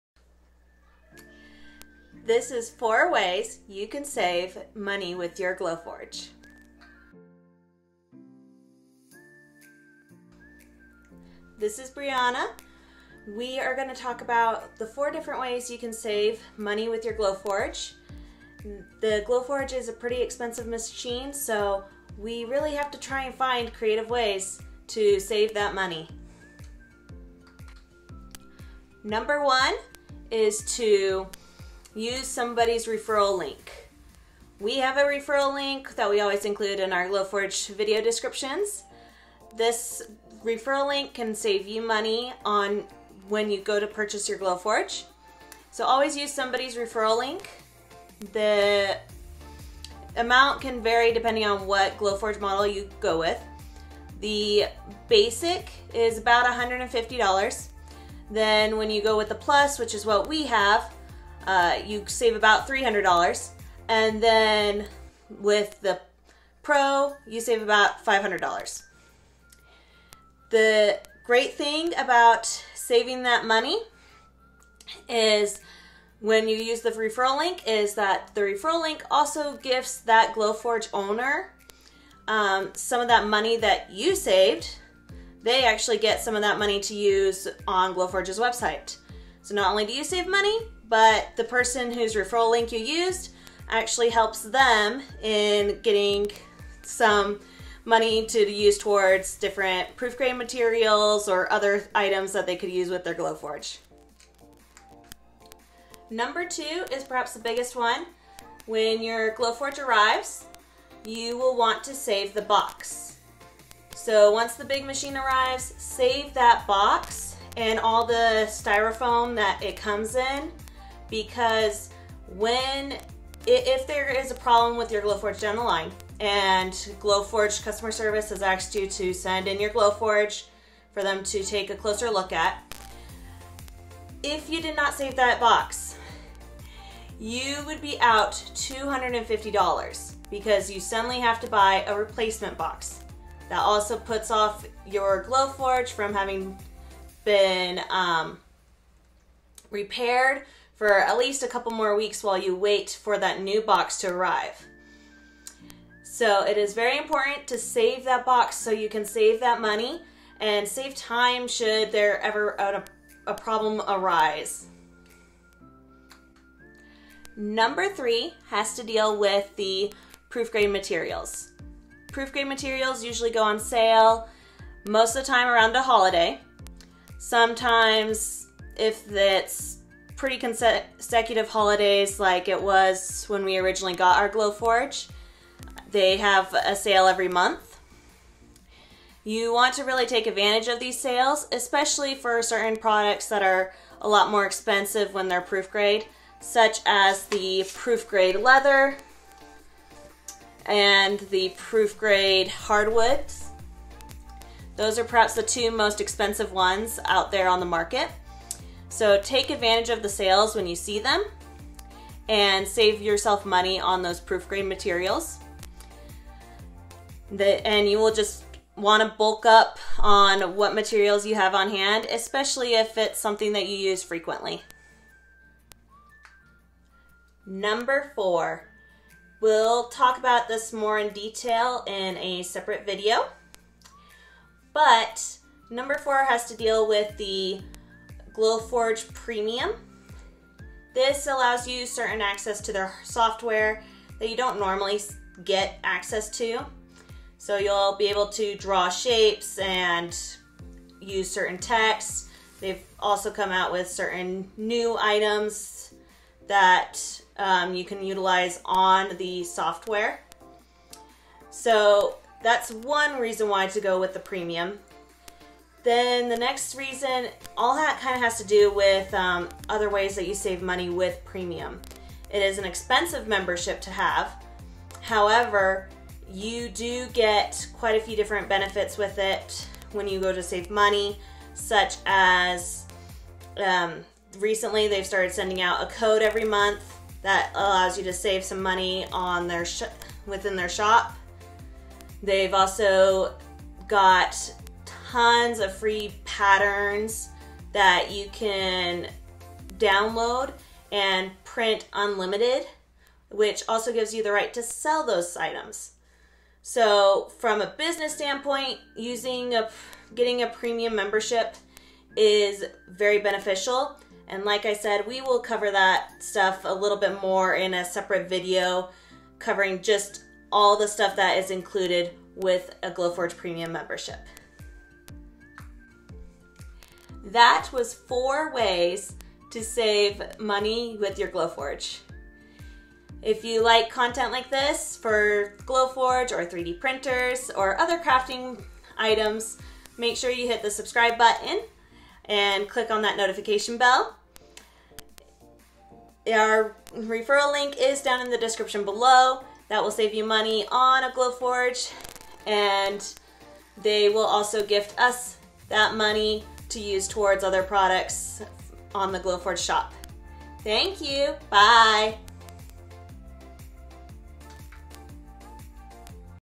4 Ways to Save Money with Your Glowforge | Glowforge Basics
In this Glowforge Basics, Brianna explains the four ways she has found to save money with your Glowforge.